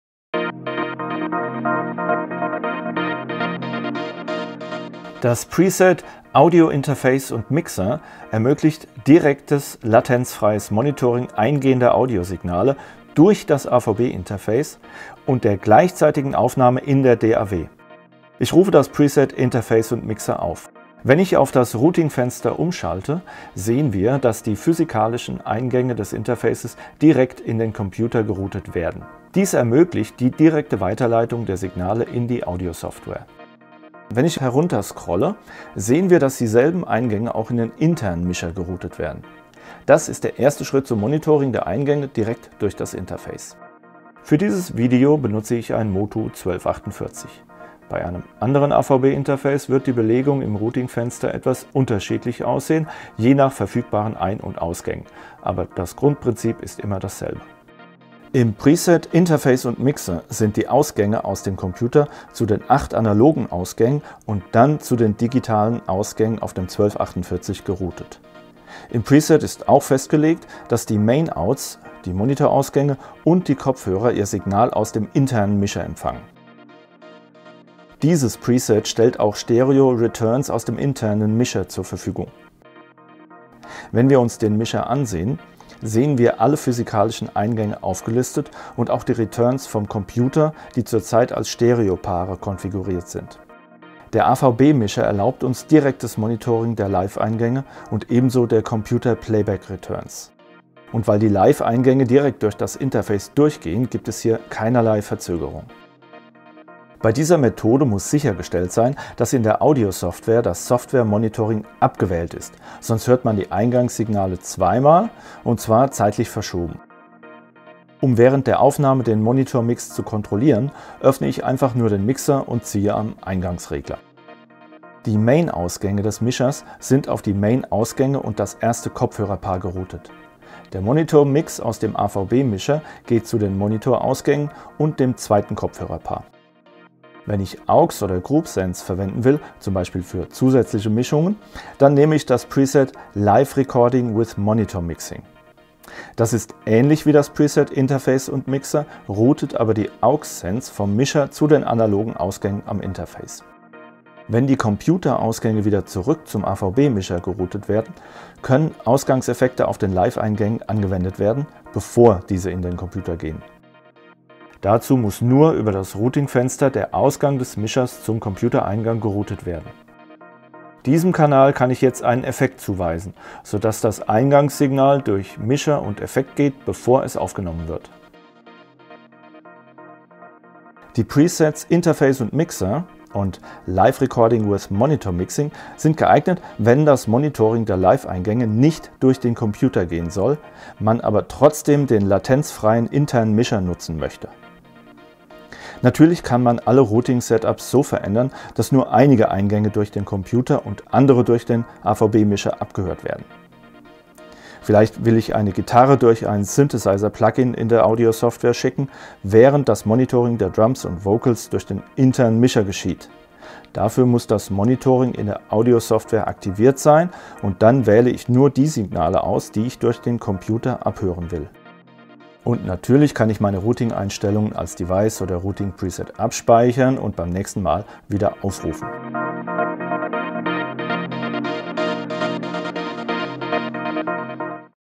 MOTU AVB Interfaces - Preset Interface + mixer
MOTU AVB Interfaces werden mit 8 Presets ausgeliefert, die direkt am Gerät oder in der AVB-Web-App ausgewählt werden können.
Das Preset Interface + mixer ermöglicht direktes latenzfreies Monitoring eingehender Audiosignale durch das AVB Interface, und der gleichzeitigen Aufnahme in der DAW.